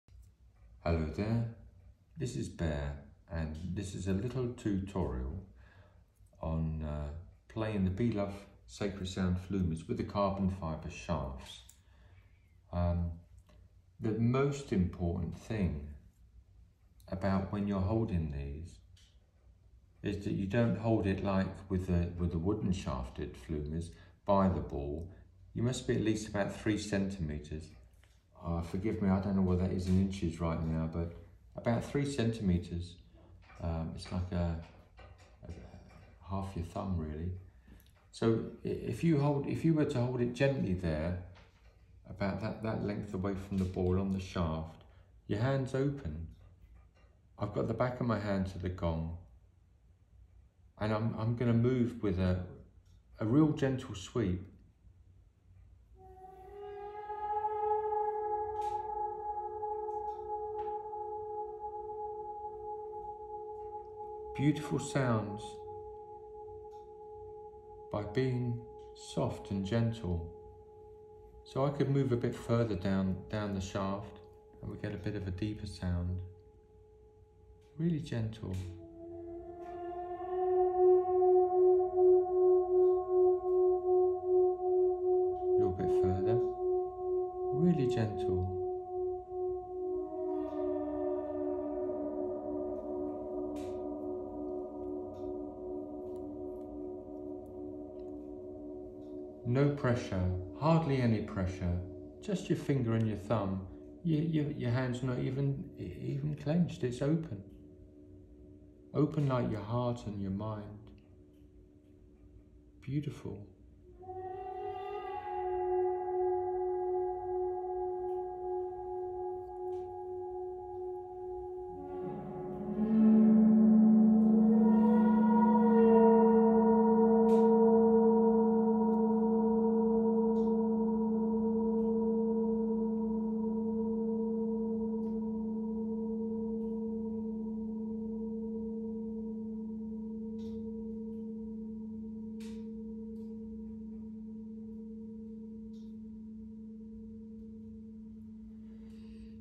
How to Use the High and Super-High sets of Professional Flumies
The B Love Sacred Sound range of professional flumies offers a rich and wide spectrum of sound textures, but most of all they offer a completely new style of gong playing. These flumies are high quality hand made, bespoke sound tools, which require careful handling.

For best performance and beautiful sound qualities sensitive playing is paramount. The sound arises just by ever so gently dragging the flumies across the gong surface. In fact no force is required at all to create powerful, voluminous sounds as well as the most profound and deep reaching sound-scapes and harmonies.

A good indication of your playing technique is how much the gong moves. If it moves a lot you're pushing too hard. the sound quality will suffer and you may damage your flumies. Problems with flumie heads coming of the shaft or splitting is invariably always a result of using too much force.

Please regularly refer back to my tutorial videos on youtube and my website, or even better come to one of my courses!